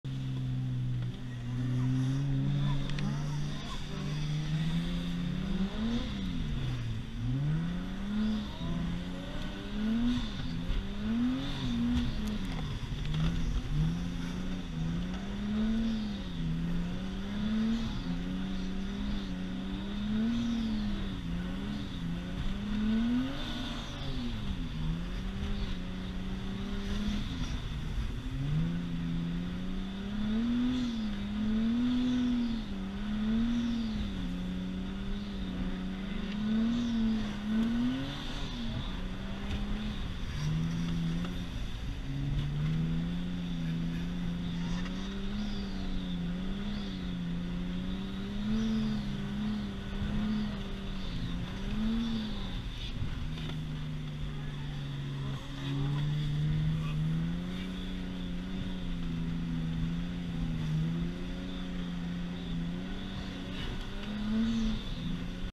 Chasing Soarer
Nissan stagea chasing toyota soarer drifting